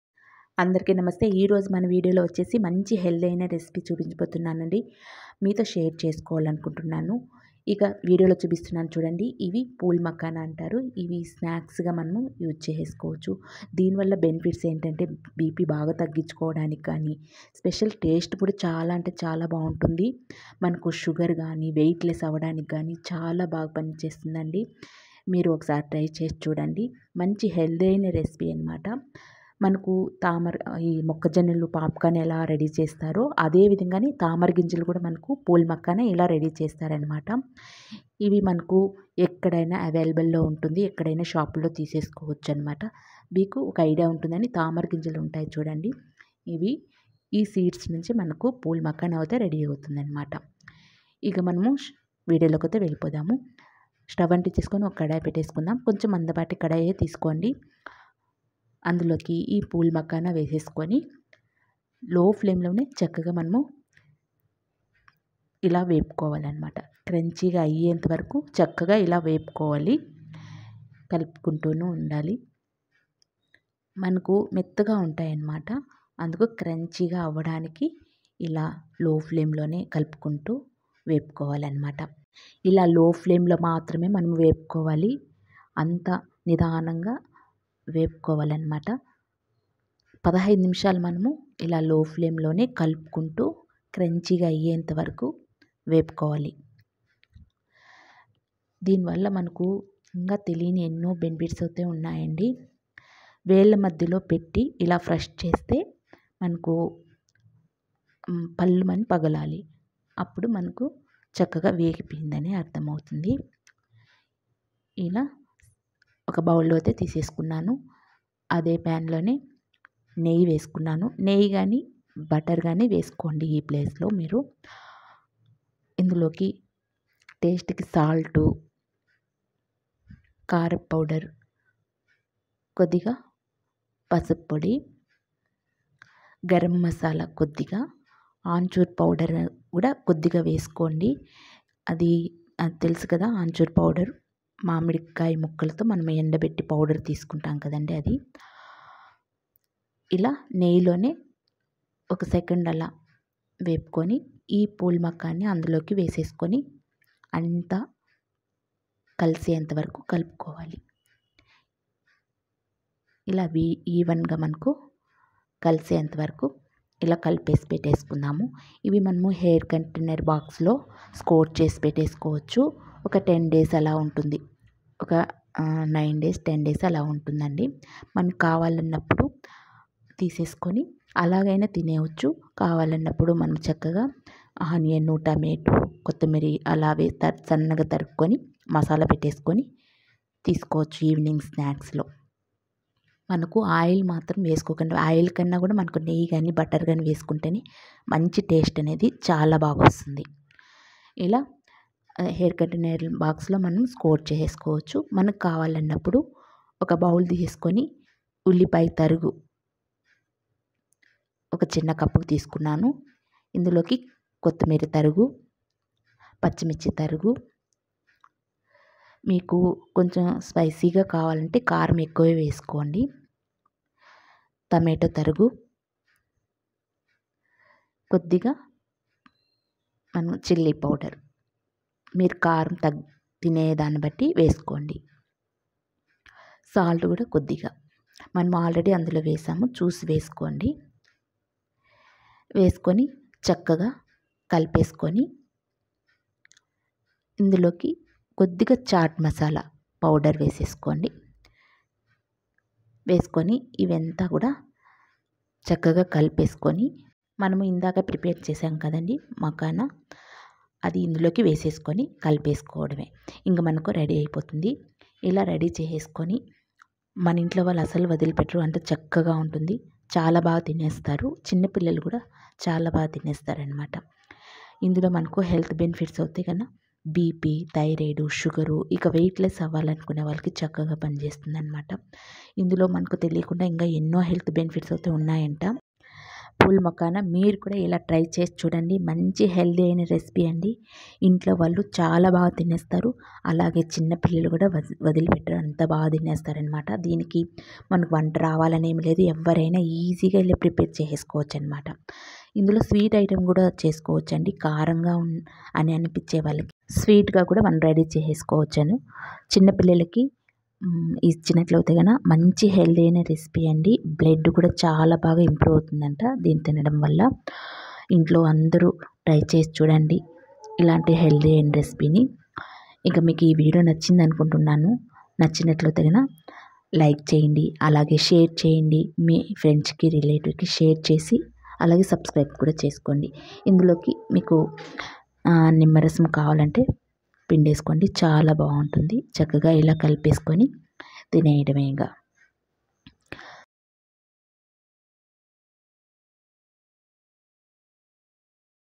పూల్ మఖాన తో ఎన్నో ఆరోగ్య ప్రయోజనాలు ఉన్న ఈ స్నాక్స్ 👈😋//phool makhana healthy recipe
పూల్ మఖానా తో ఎన్నో ఆరోగ్య ప్రయోజనాలు ఈ స్నాక్స్//phool makhana healthy recipe